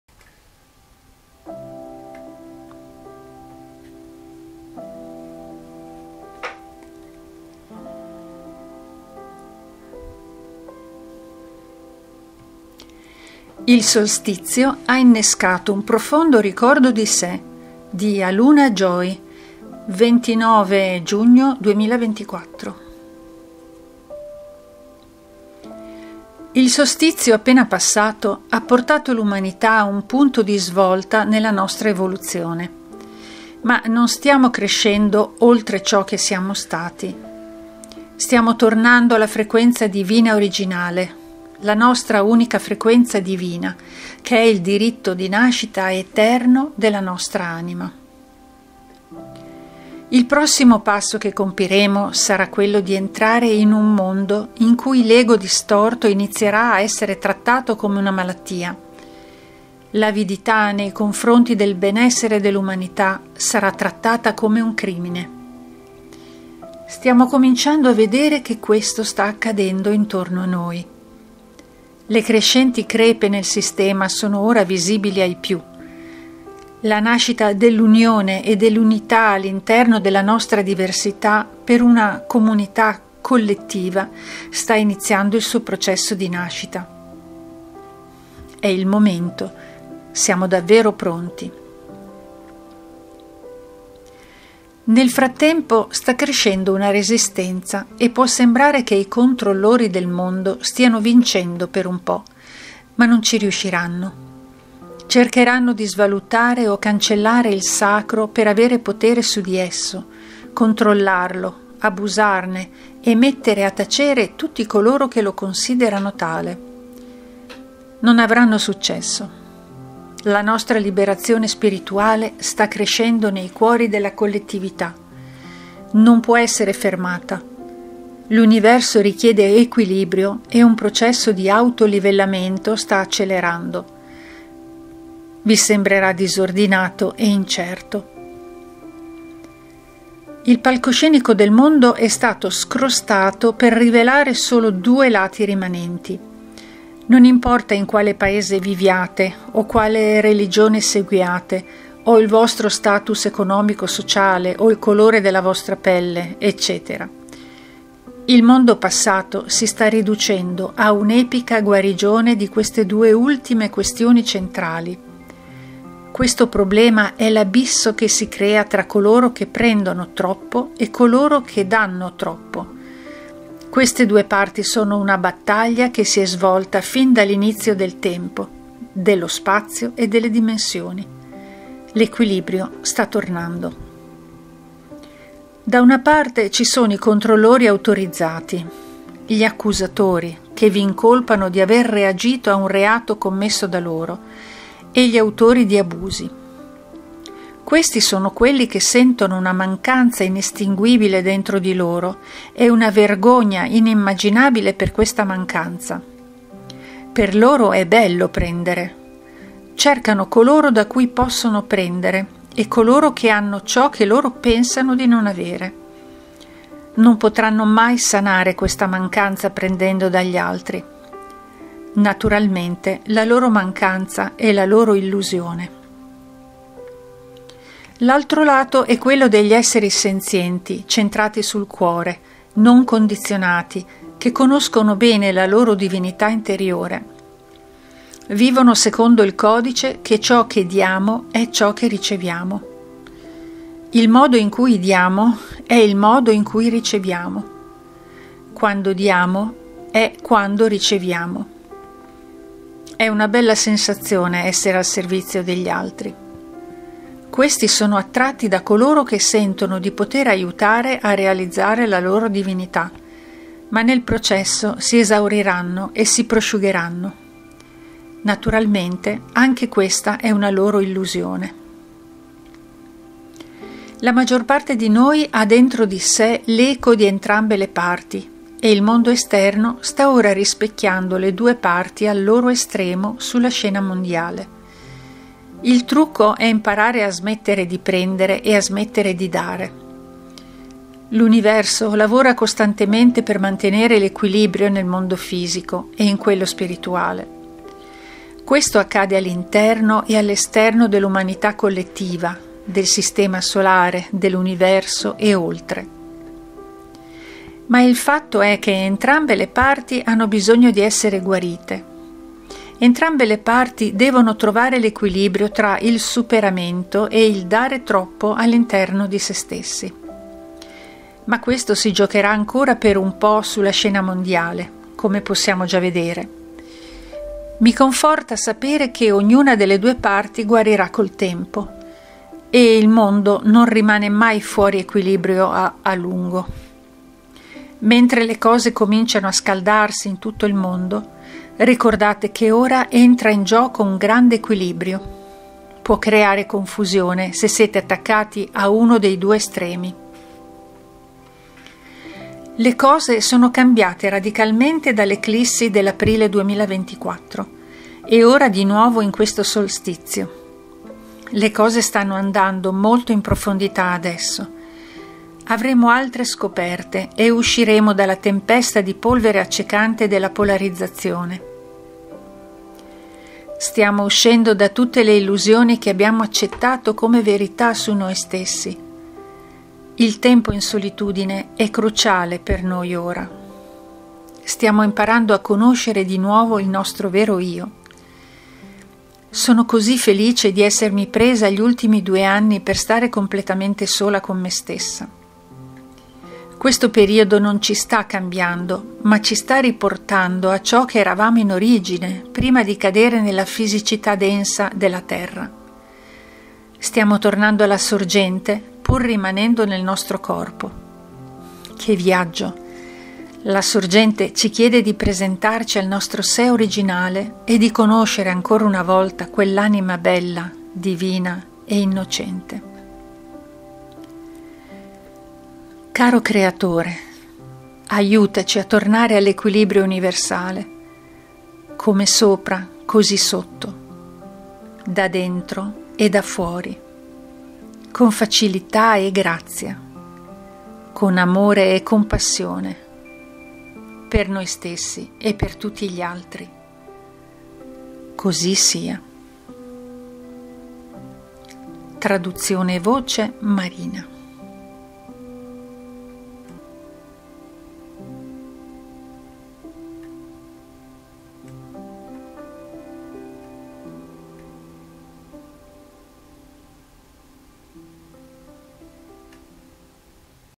Il Solstizio ha innescato un Profondo Ricordo di Sé, di Aluna Joy, 29/06/2024
Traduzione e voce Marina.

Il solstizio appena passato ha portato l'umanità a un punto di svolta nella nostra evoluzione. …

Musica by Deep Instrumental.